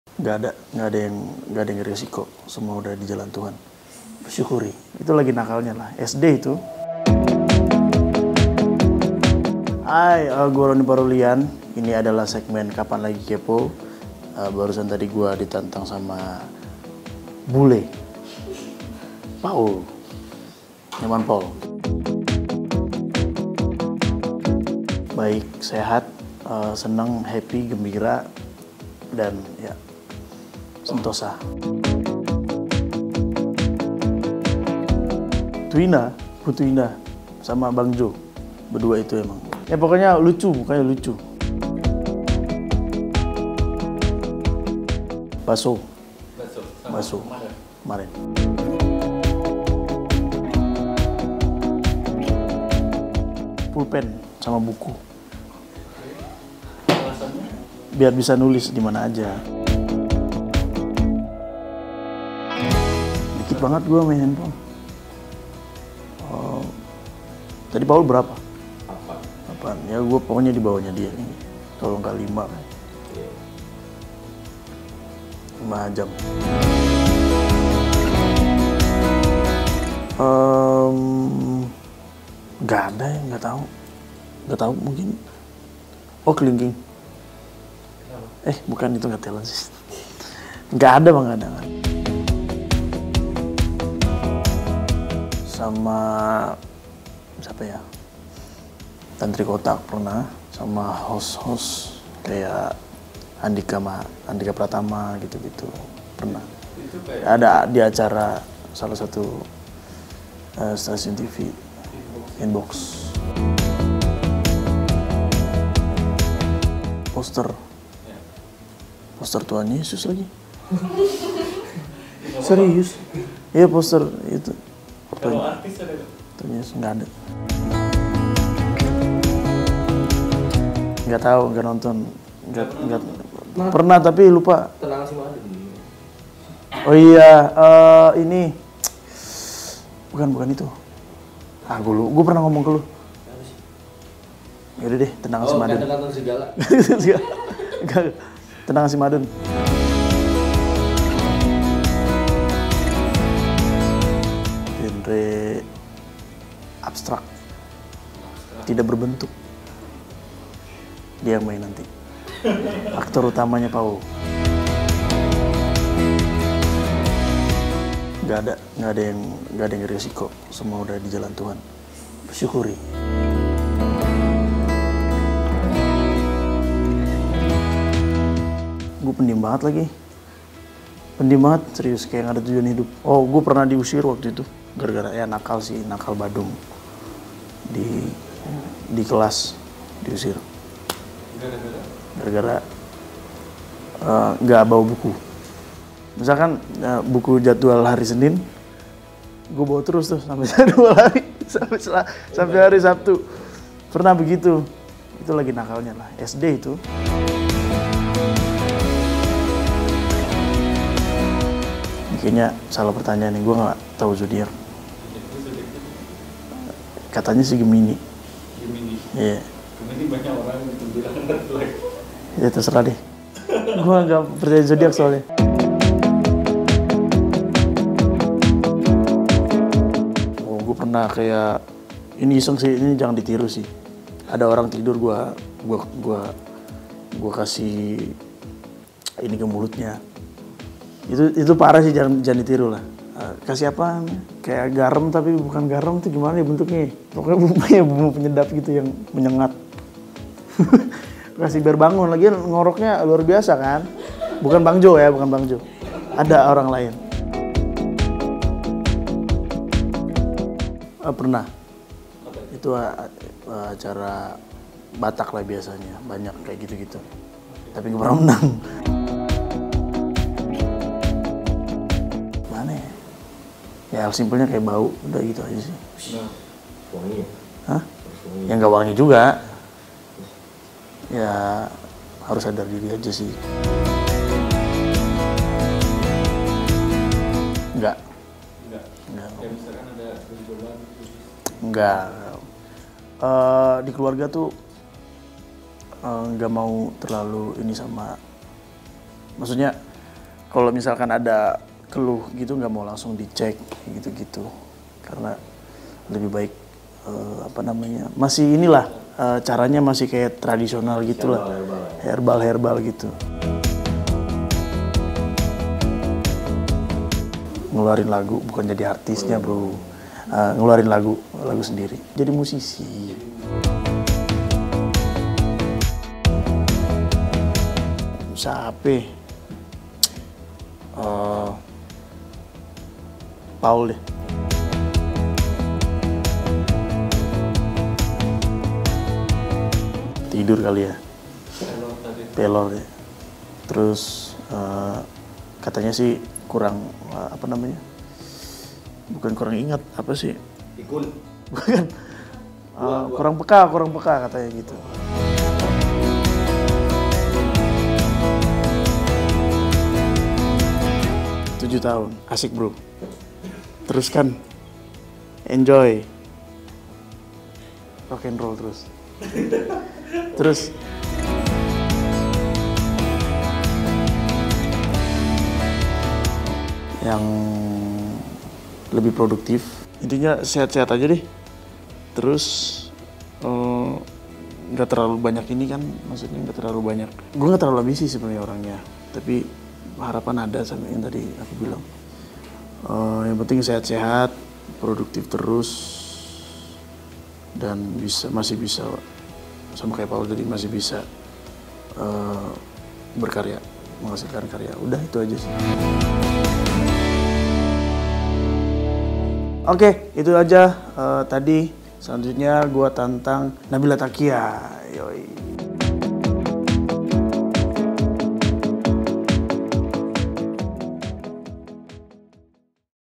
Kepo Ronny Parulian, Dari Zodiac Sampai Pasangan Redflag
KAPANLAGI.COM - KapanLagi juga menantang Ronny untuk menjawab pertanyaan KapanLagi Kepo. KLovers mau tau gimana keseruan ceritanya? Yuk kita saksikan pada video berikut ini!